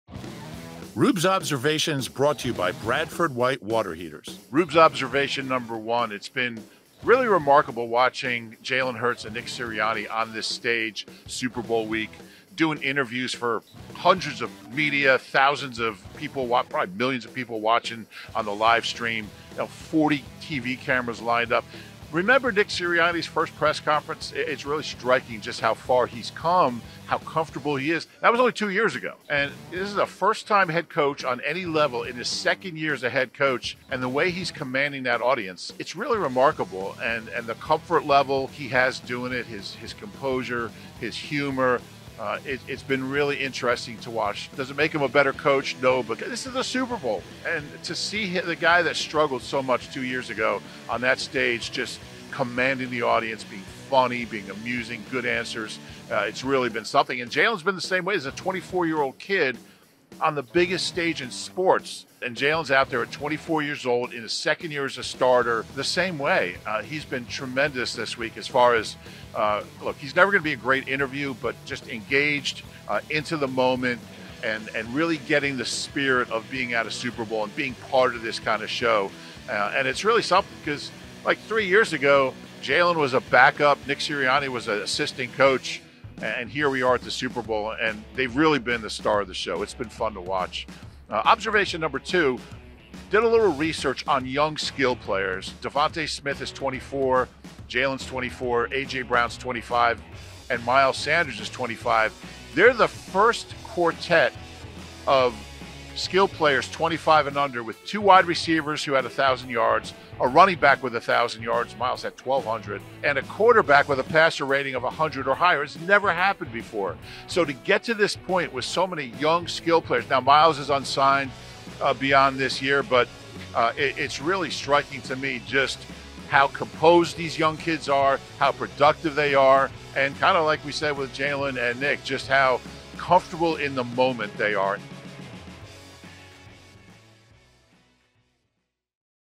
Young Eagles playmakers made history this season | Roob's Observations
Reuben Frank shares his observations from the week leading up to Super Bowl LVII including a look at Jalen Hurts' and Nick Sirianni's composure during the media circus and a nugget on the Eagles' production from the skill positions in 2022.

» Download the MyTeams app to watch live games and highlights:
iOS: